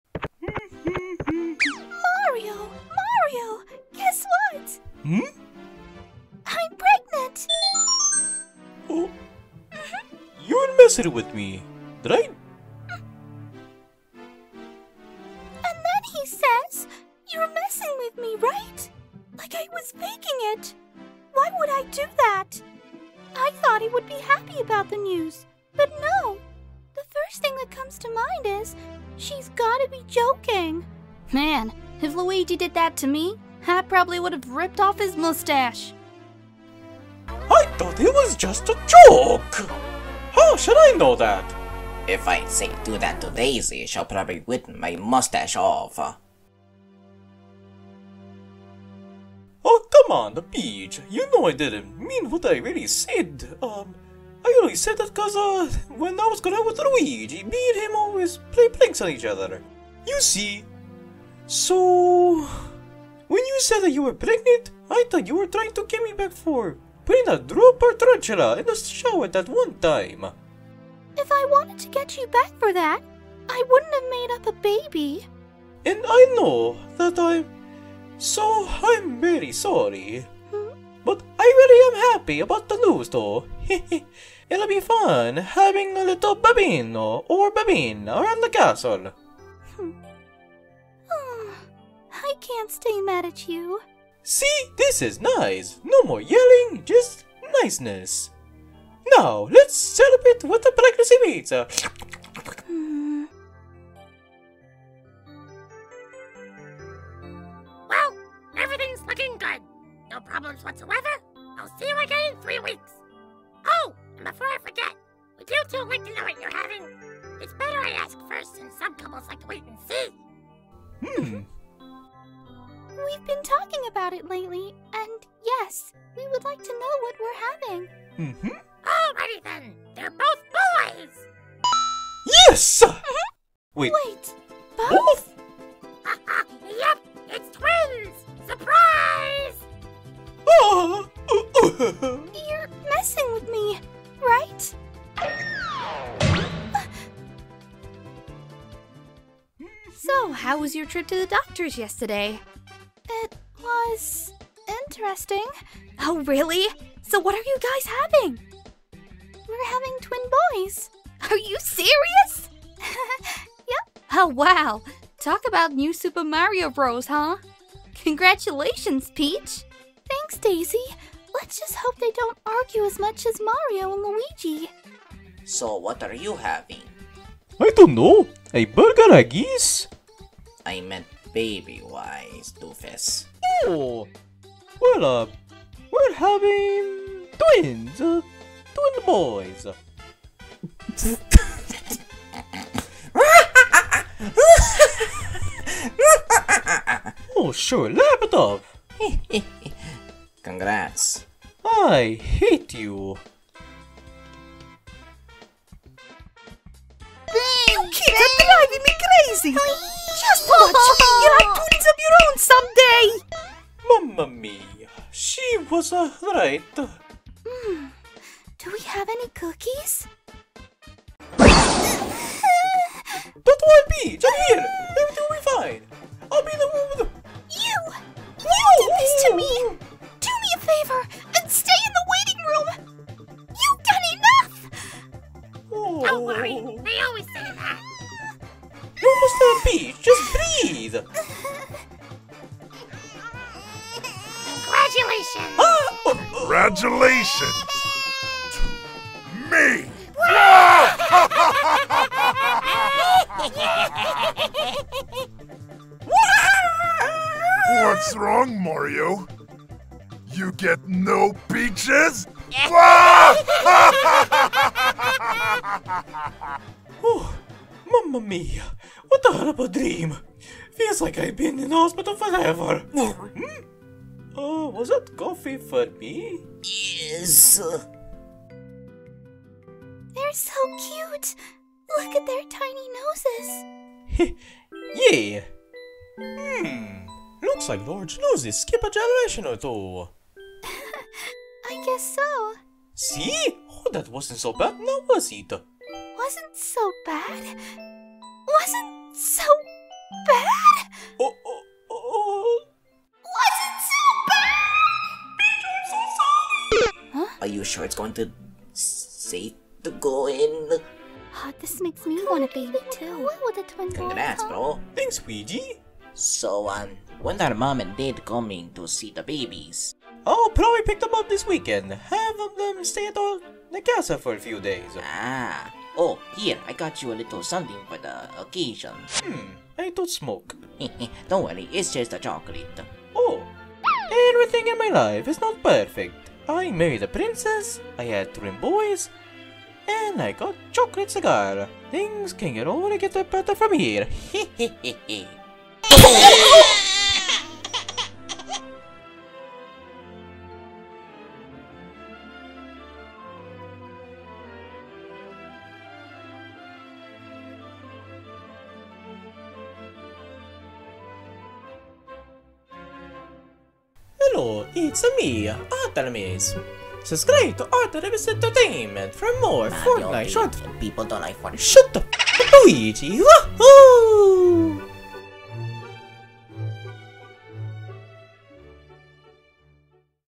Mario & Peach Have a Baby (Comic Dub)
Thanks to the very talented artist and voice actors featured in this video! Let's give Nintendrawer thanks for letting us dub your comic!

Stay rare like a Diamond!